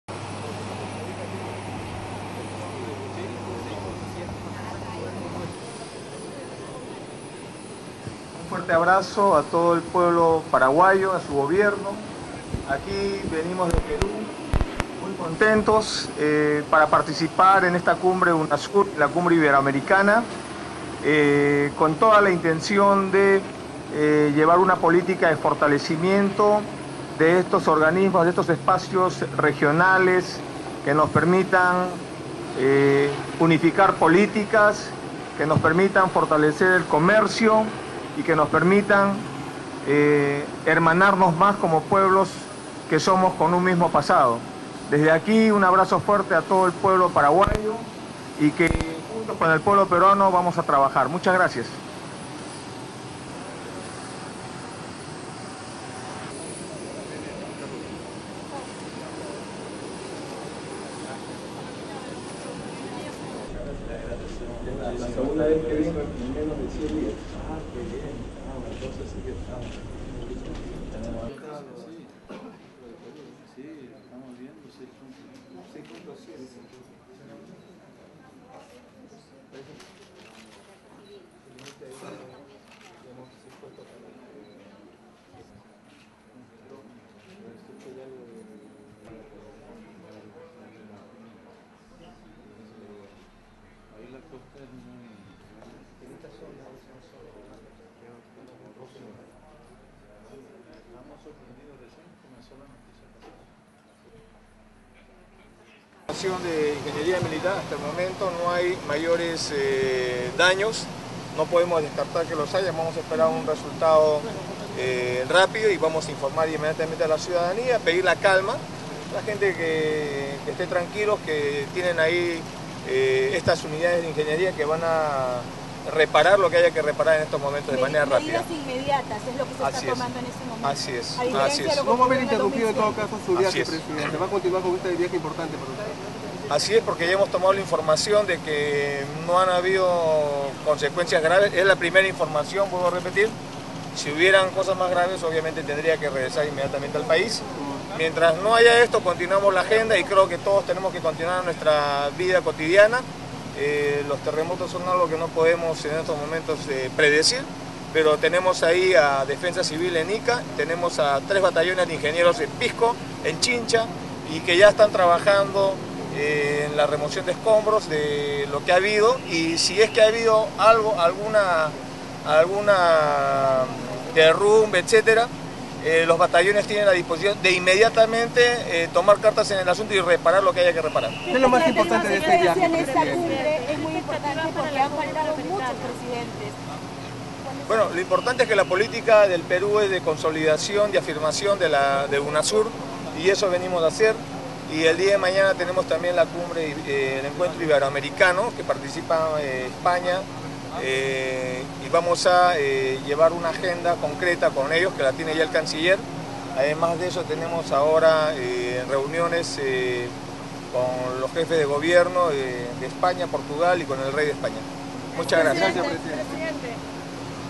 Presidente Ollanta Humala inició sus actividades en Paraguay
El presidente de la República, Ollanta Humala Tasso, inició esta tarde sus actividades en la ciudad de Asunción donde participará de la XXI Cumbre Iberoamericana de Jefes de Estado y de Gobierno.

El jefe de Estado arribó al Aeropuerto Internacional Silvio Pettirossi a las 16.30 horas (14.30 horas en Lima) y fue recibido por Director General de Protocolo de Paraguay, Oscar Benites  y el Embajador de Perú en ese país, Jorge Lázaro.